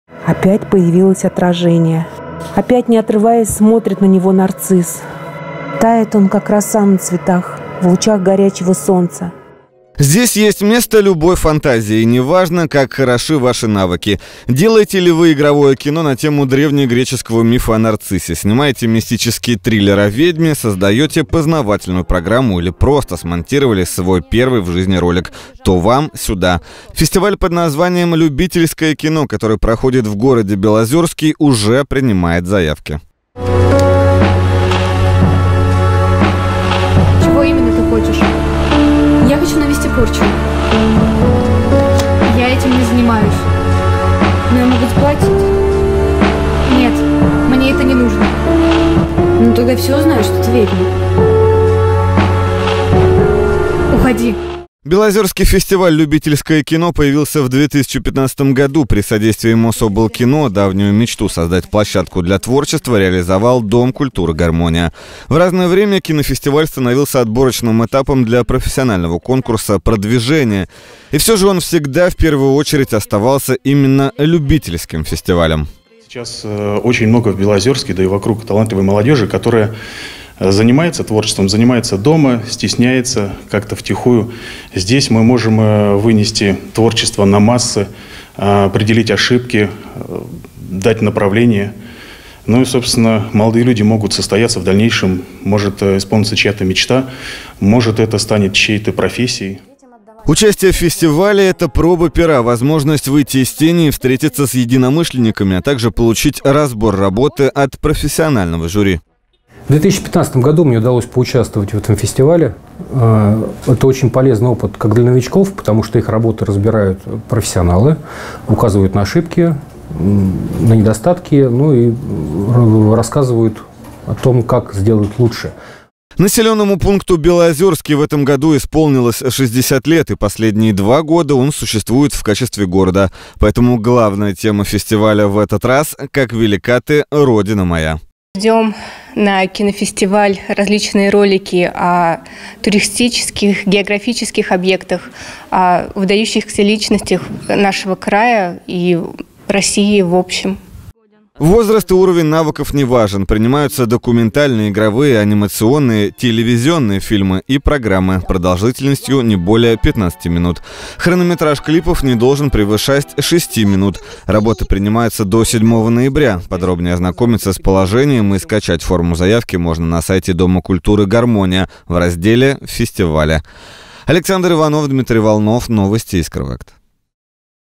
Мотор! Белоозерский фестиваль «Любительское кино» принимает заявки
Создавать контент сегодня можно без профессионального оборудования полагаясь лишь на смартфон. А вот как проверить свои навыки? Седьмой белоозерский открытый фестиваль «Любительское кино» предлагает не только конкурентную среду, но и общение с профессионалами. Что нужно делать, чтобы стать участником, расскажем в материале.